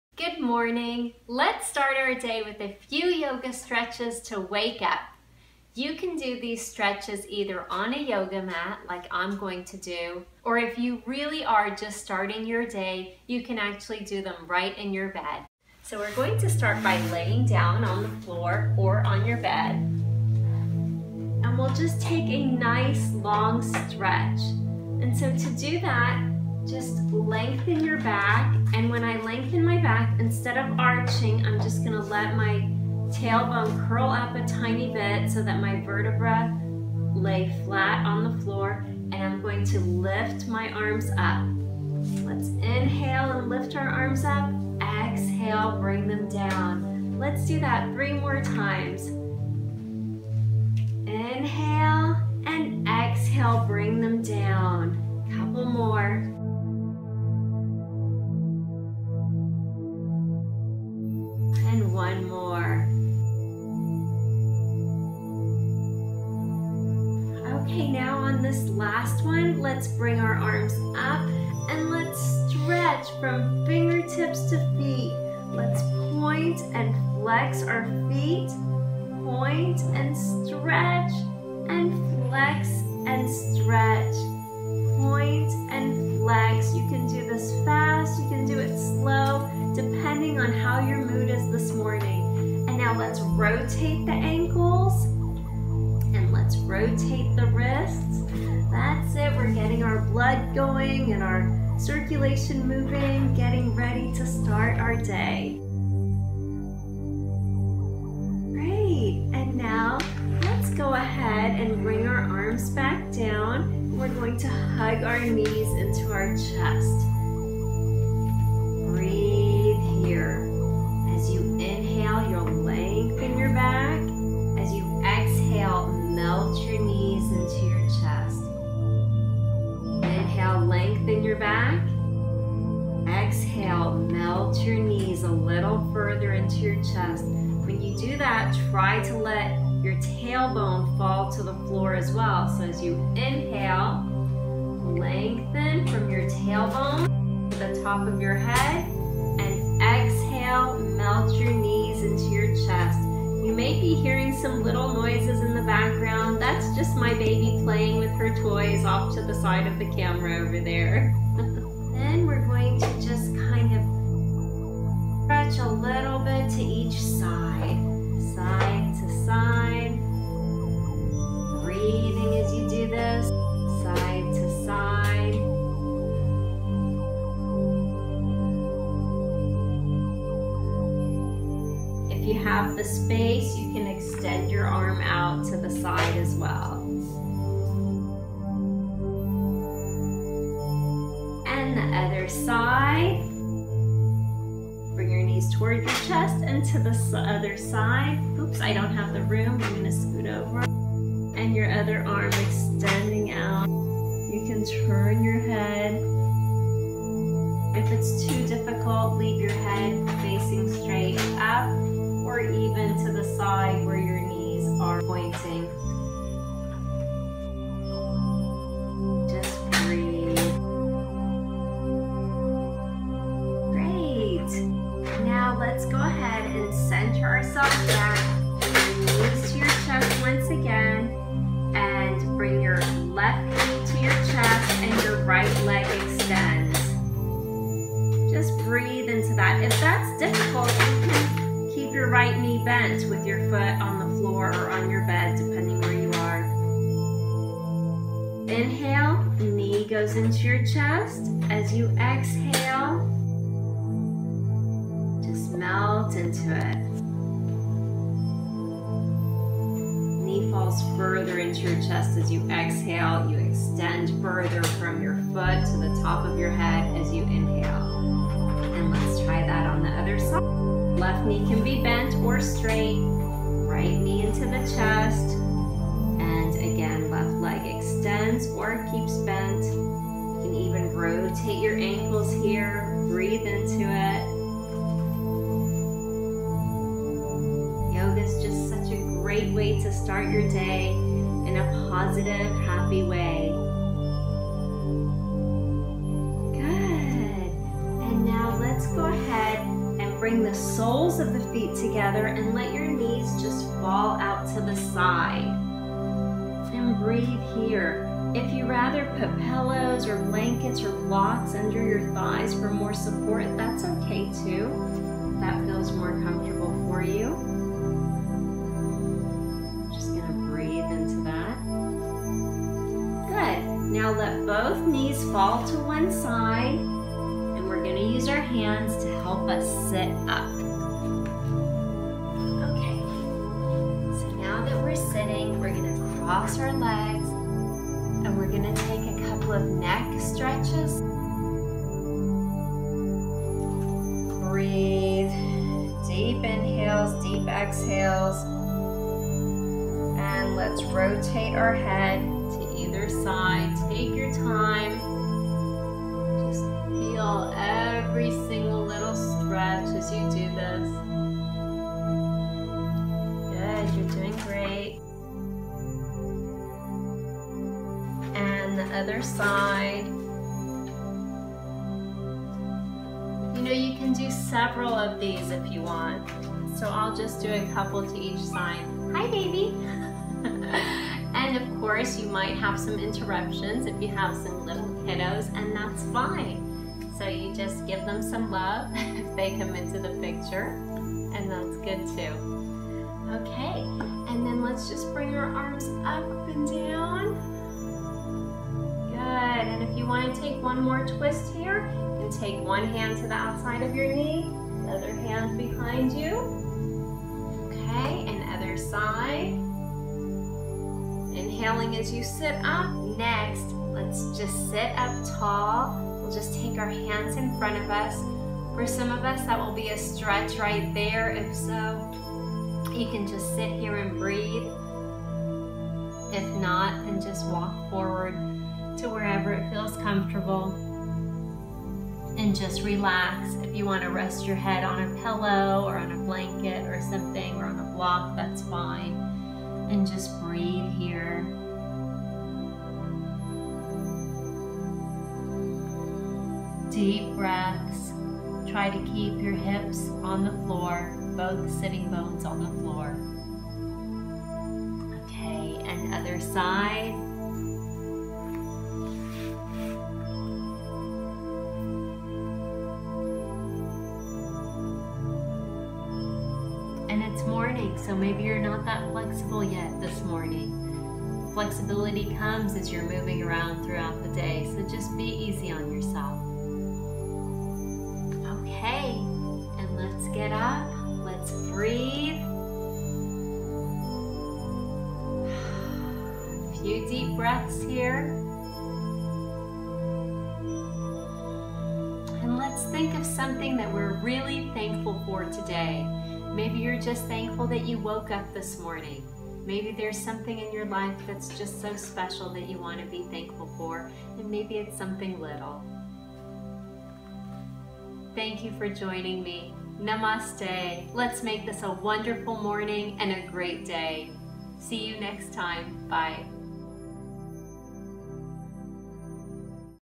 Beginner Yoga - Gentle Easy Yoga
Beginner Yoga - Gentle Easy Yoga. 8 Minute Beginners yoga for morning, Gentle Easy Yoga for all Levels. Yoga to start your morning happy, calm & relaxed. Easy gentle yoga routine.  Get centered & practice this work out exercise from your bed before you even get up.  My sweet baby toddler even helps. Yes, life gets in the way. Let's all just do the best we can to breathe and get centered.
This practice can be done to energize your morning or to relax you in the evening.
Nemaste